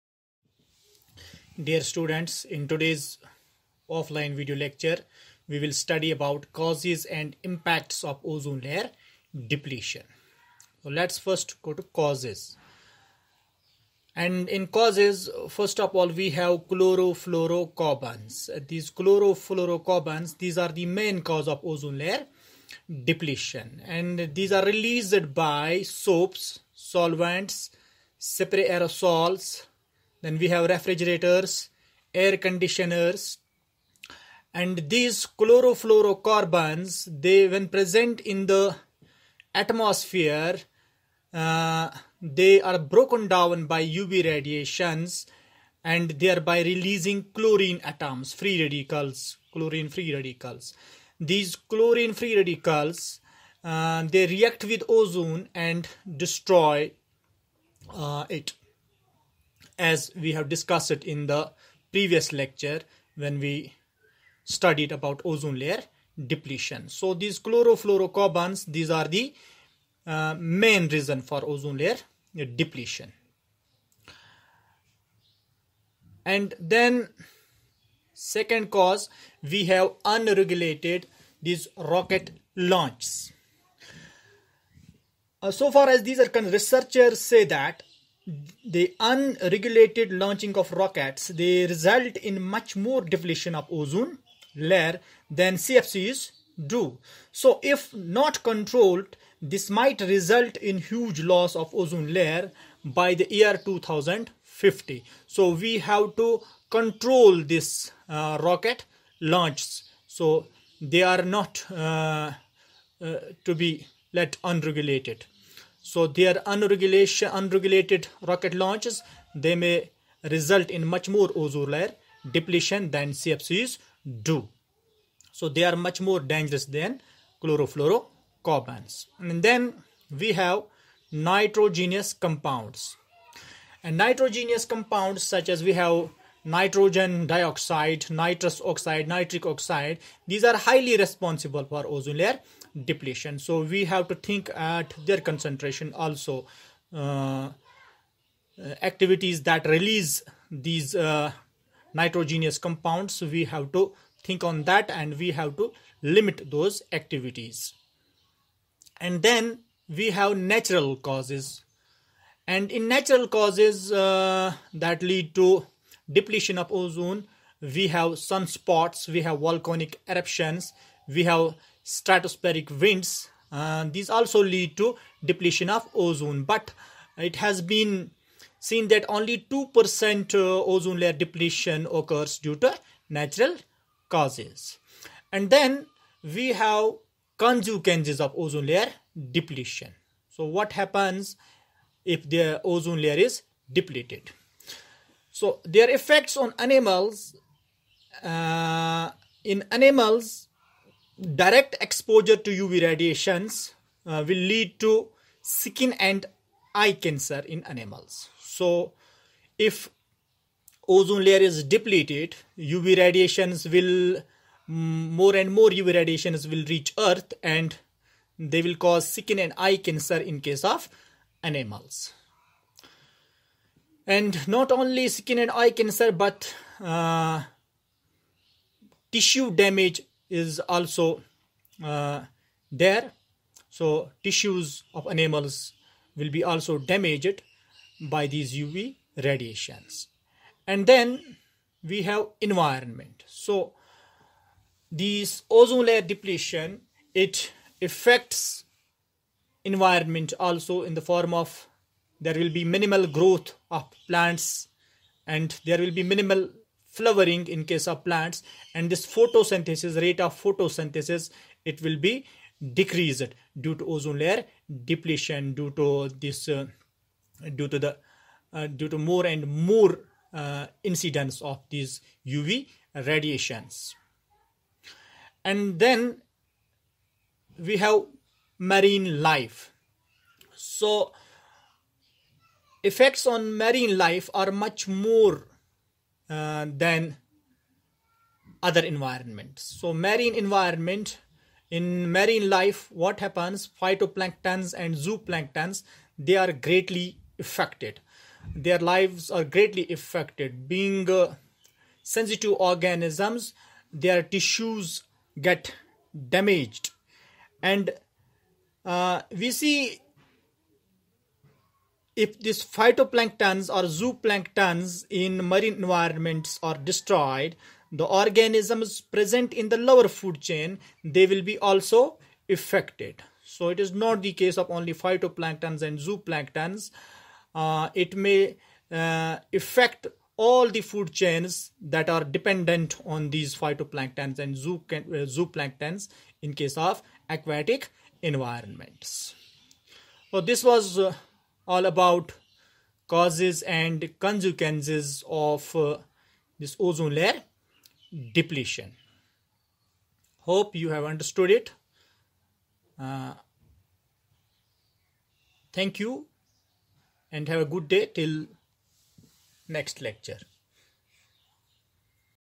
Causes and effects of ozone layer depletion
Class 12th Environmental Science